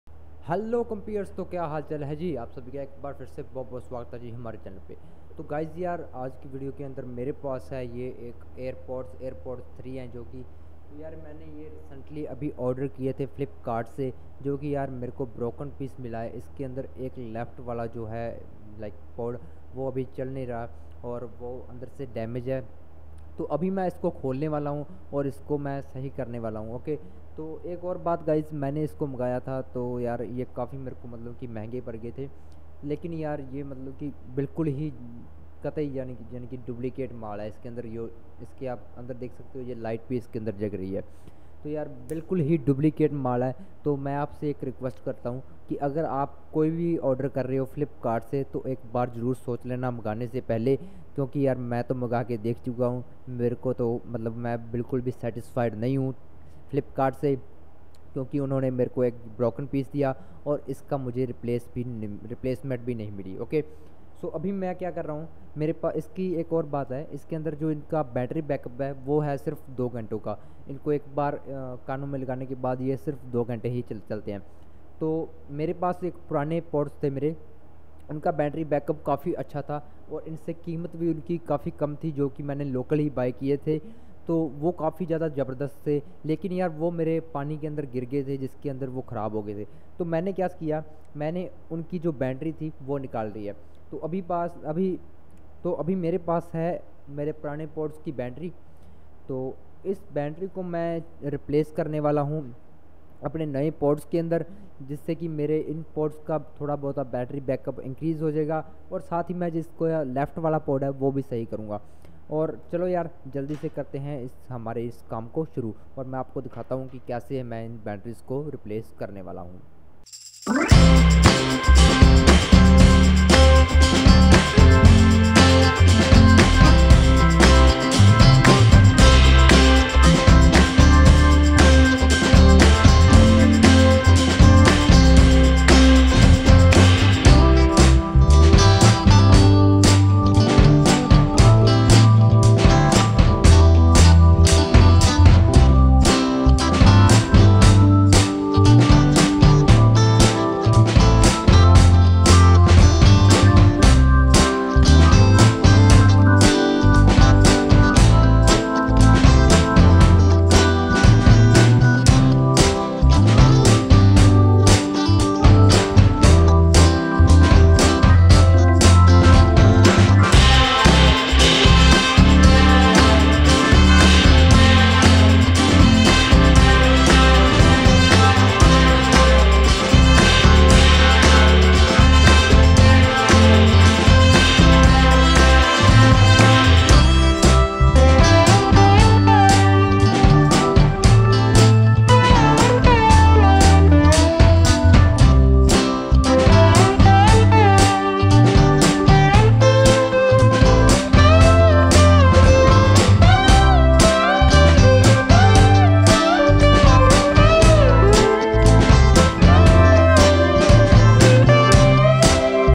repairing AirPod’s 3 || i ordered airpod’s from flipkart || how to rebrand your airbirds || OpJatt
sahaj_sandhu